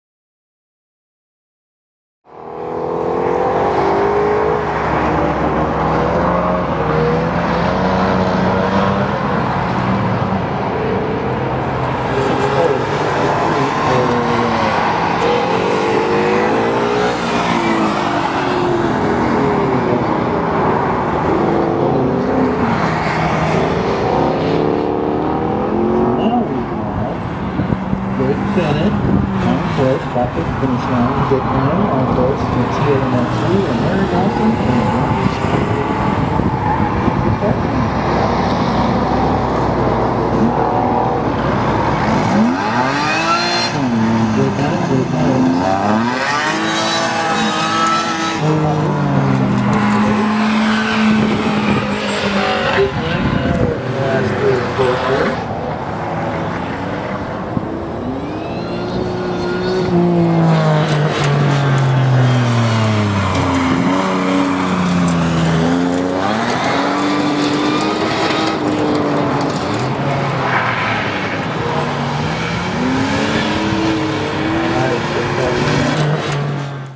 6_2_2012 EESCC Autocross 28ES — 1993 Toyota MR2
Emerald Empire Sports Car Club Autocross #3 in Coburg, OR. 1993 Toyota MR2